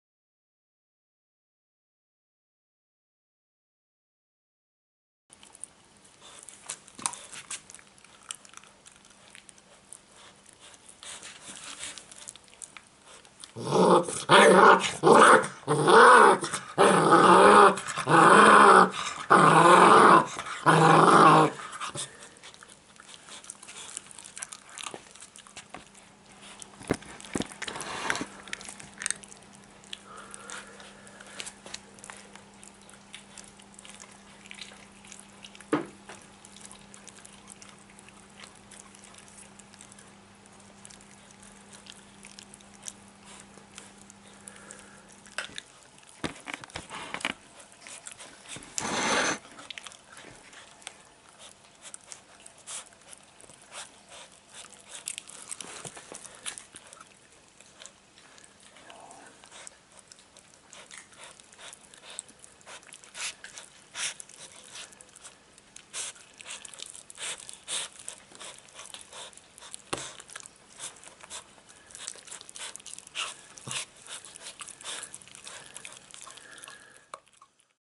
2017 07 09 Csenge és Cudar nasi Molnár István Dunaújváros
Csenge és Cudar a két hat hetes kuvasz kölyök.
Nem lehet elég korán elkezdeni az étellel kapcsolatos szocializálást. Molnár István Dunaújváros